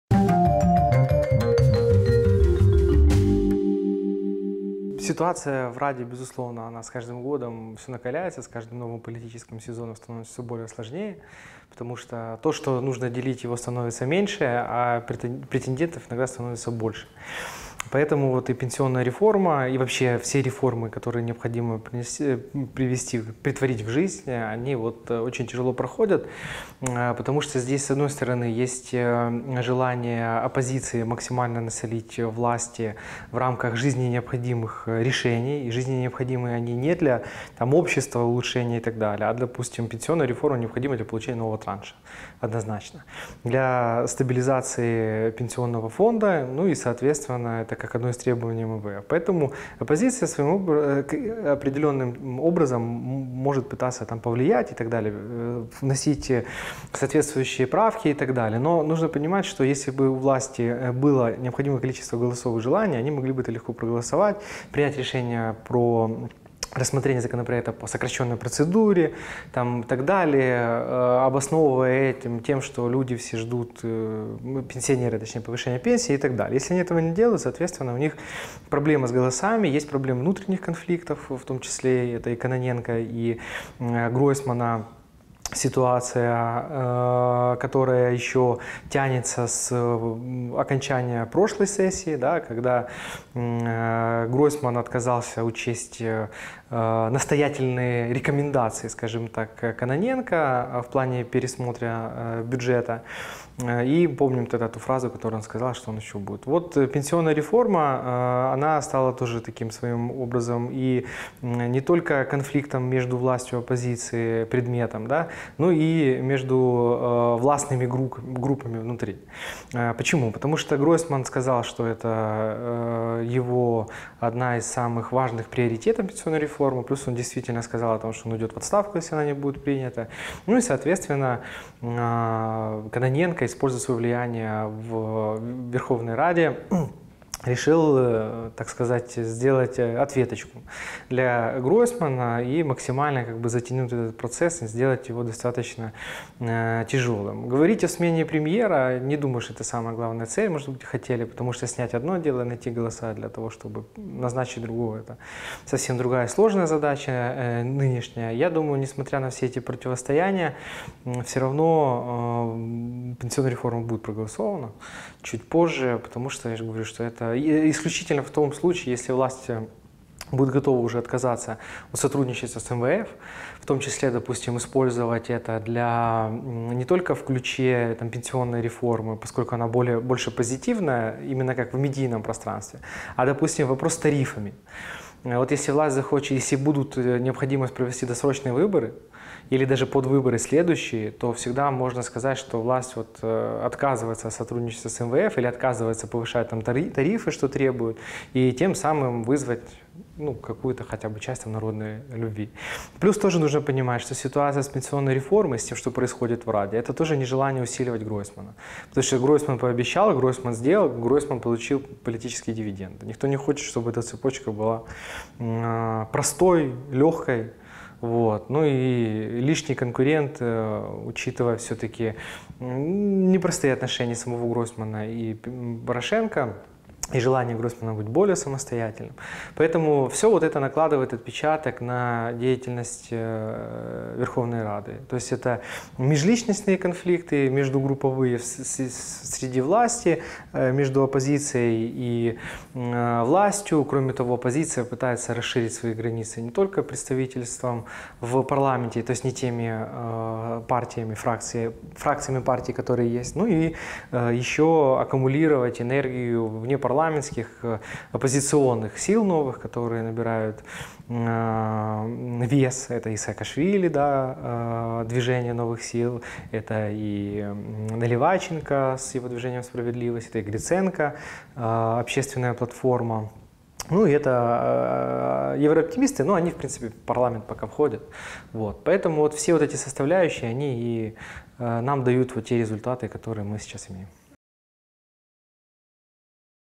О Гройсмане и перипетиях в Раде вокруг пенсионной реформы
Эксперт Международного центра перспективных исследований Игорь Петренко о перипетиях в Верховной Раде вокруг принятия пенсионной реформы